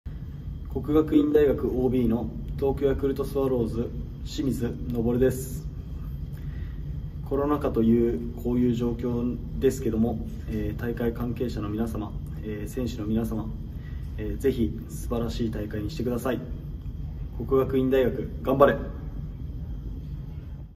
【プレユニ22 4/2開幕！】OBメッセージ from 清水昇(東京ヤクルトスワローズ/國學院大学OB)
4月2日開幕！
東都大学野球春季リーグ戦を前に、プロの世界で戦うOBたちから現役選手へメッセージが届きました！

清水昇(東京ヤクルトスワローズ/國學院大学OB)
「コロナ禍という状況ですが、大会関係者の皆様、選手の皆様、ぜひ素晴らしい大会にしてください！」

🐃リーグ戦ライブ配信はコチラ
スポーツブルでは東都大学野球春季リーグ戦全試合をライブ配信！